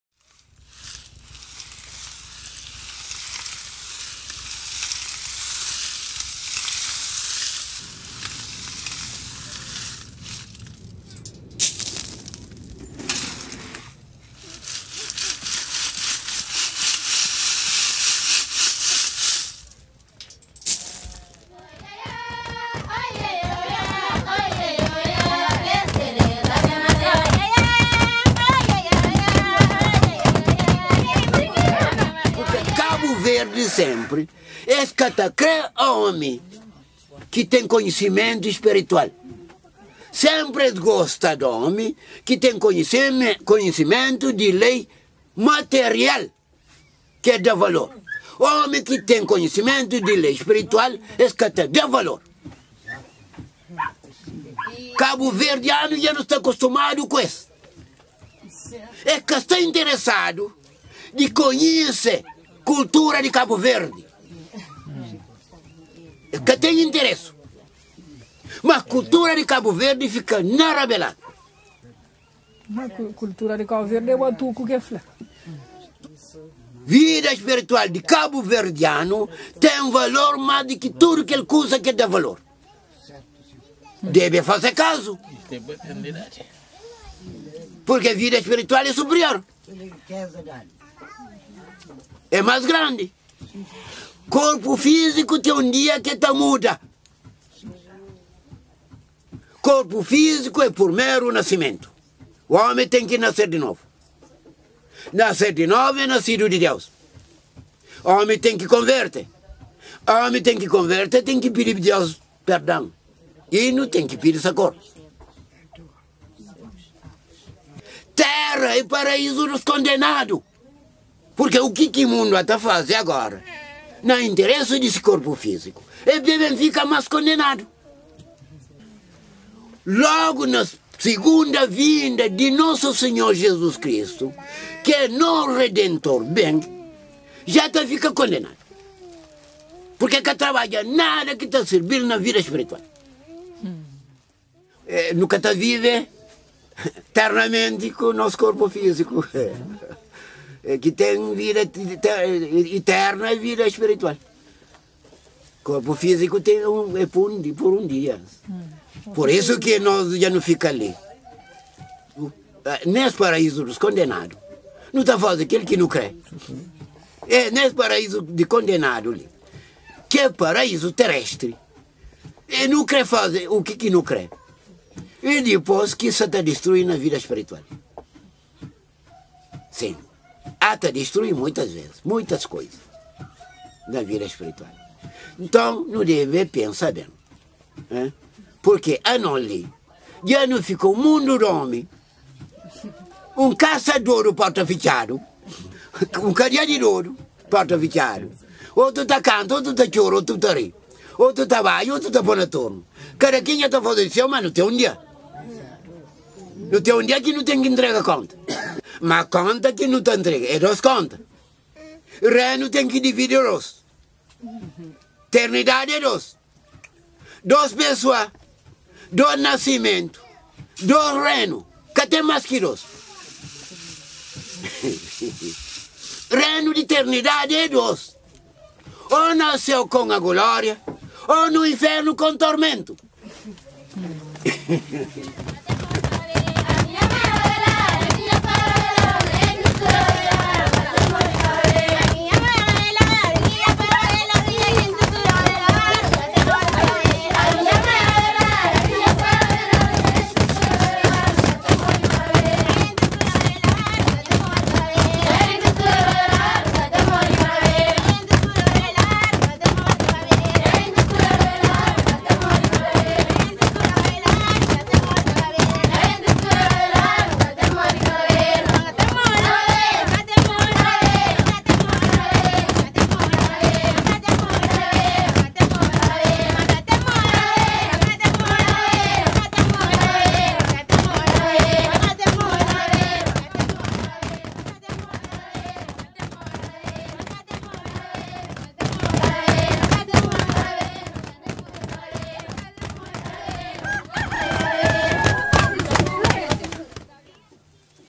Le développement du Cap-Vert ne sera pas possible sans un retour aux pratiques ancestrales.
De passage dans l'île de Santiago au Cap Vert, découvrez le village d'Espinho Branco et la communauté des Rabelados. Vous découvrirez alors un village artisanal original au coeur des montagnes, mais vous irez surtout à la rencontre d'un peuple unique à l'histoire marquée de souffrances et d'espoir.